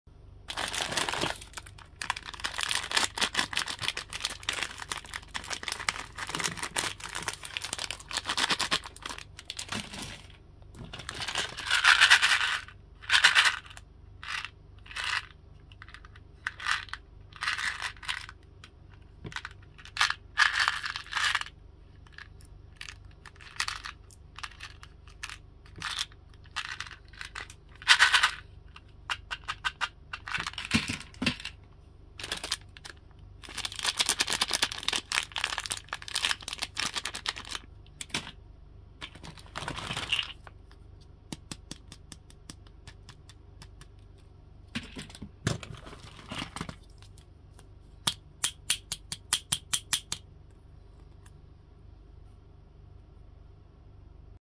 🍬🍭 ASMR Satisfying Video | Yummy Glitter Rainbow Lollipops Unpacking - A Lot Of Lollipops No Talking
ASMR Satisfying Video | Yummy Glitter Rainbow Lollipops ASMR Unpacking - A Lot Of Lollipops | Unpacking candies | japanese candies